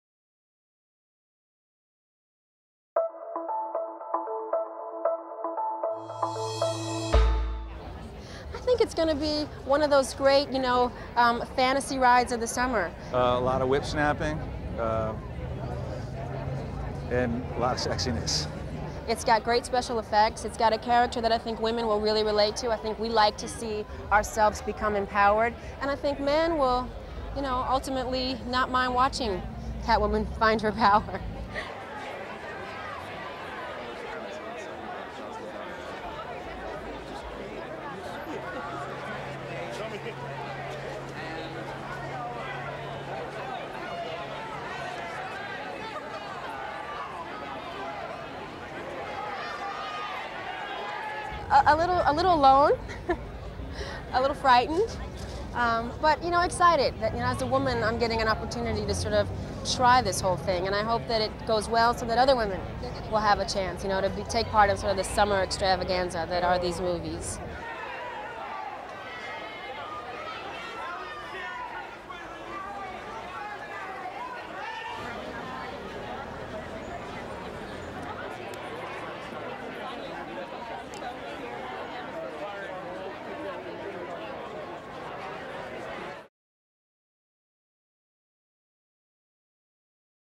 On This Day: 19 July 2004
OPENING CREDITS CONSIST OF AP TELEVISION/BRITISH MOVIETONE/AP IMAGES COPYRIGHT MATERIAL"
ASSOCIATED PRESS
Los Angeles, 19 July 2004
1. SOUNDBITE (English) Halle Berry, actor:
"I think it's going to be one of those great, you know, fantasy rides of the summer."
2. SOUNDBITE (English) Benjamin Bratt, actor:
"A lot of whip-snapping, and a lot of sexiness."
3. SOUNDBITE (English) Halle Berry, actor:
"It's got great special effects.  It's got a character that I think women will really relate to.  I think we like to see ourselves become empowered and I think men will ultimately not mind watching Catwoman find her power."
4. Wide shot Benjamin Bratt and wife Talisa Soto on red carpet
5. Wide shot Venus Williams
6. Wide shot Snoop Dogg
7. Medium shot; pull-out Halle Berry waves to fans
8. SOUNDBITE (English) Halle Berry, actor:
"A little alone, a little frightened, but you know, excited that as a woman I am getting an opportunity to sort of try this whole thing and I hope that it goes well so that other women will have the chance to take part in the summer extravaganza that are these movies."
9. Wide shot Sharon Stone, Halle Berry and Lambert Wilson
10. Cutaway press
11. Medium shot Halle Berry talking to reporters